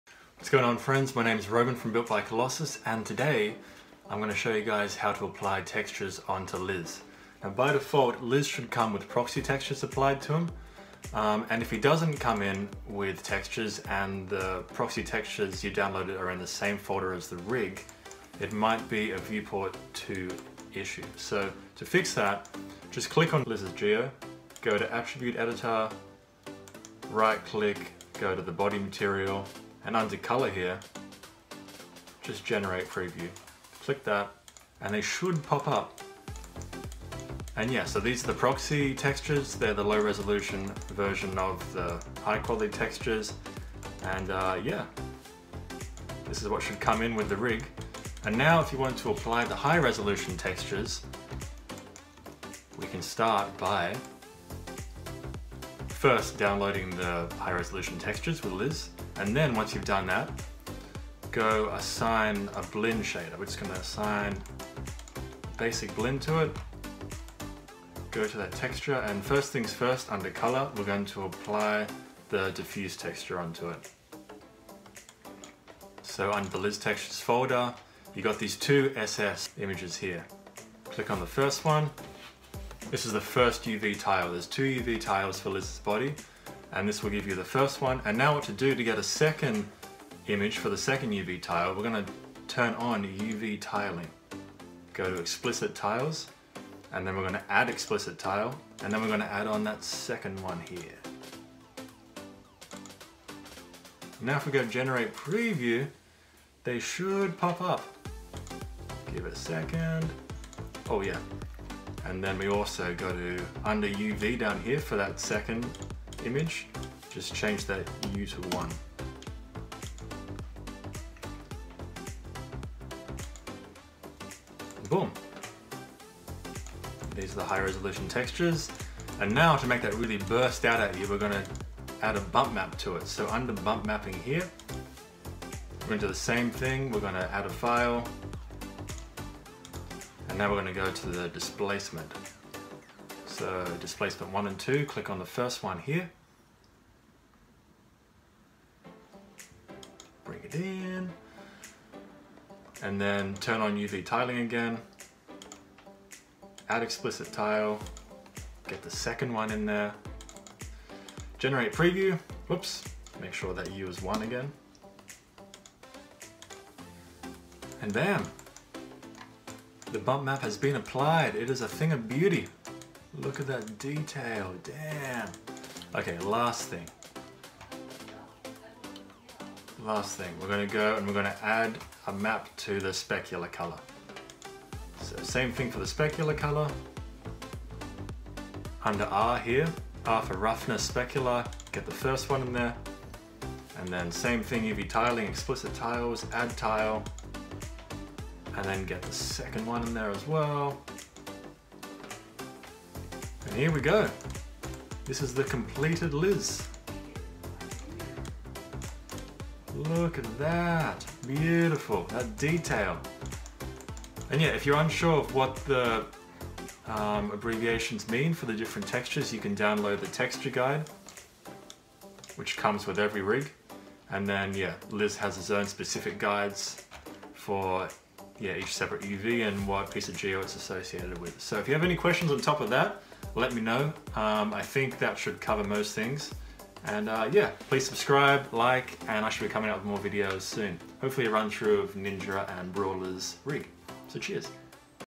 [BUILT BY COLOSSUS] APPLYING TEXTURES TO MAYA RIG LIZ
A quick demonstration on how to apply your downloaded textures to the Liz rig. :)

Happy animating!